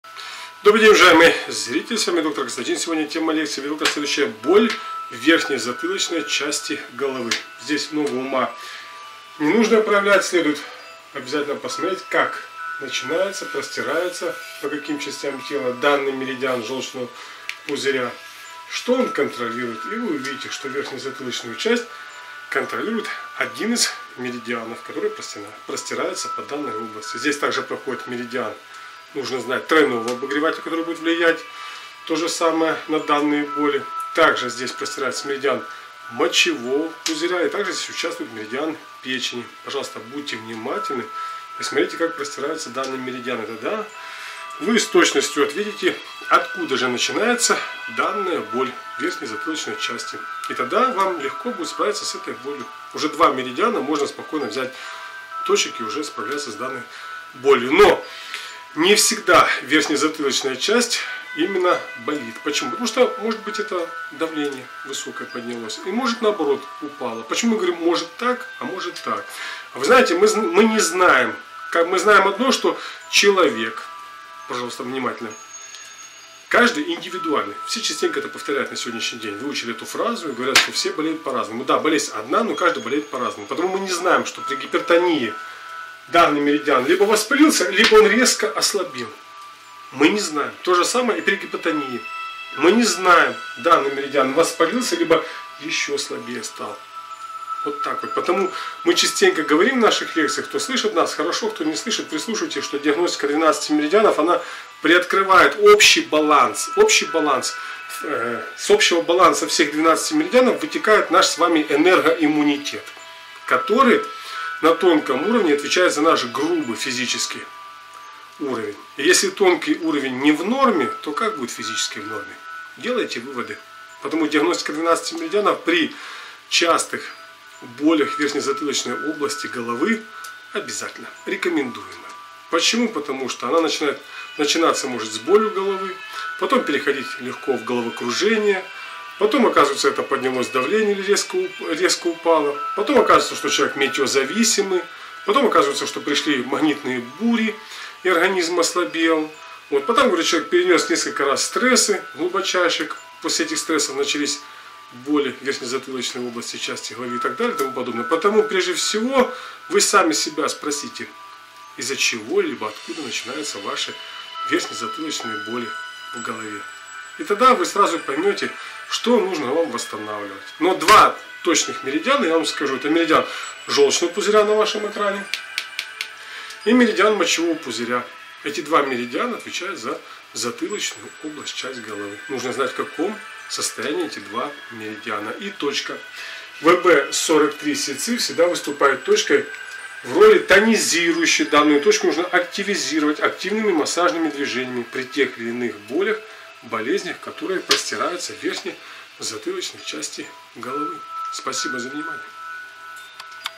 Боль в Верхнее-Затылочной части Головы. Помощь акупунктурой.
Точки акупунктуры и Состояние Меридианов в помощь при Болях в Затылочной части Головы. Видео-Лекция Иглотерапевта Доктора Константина.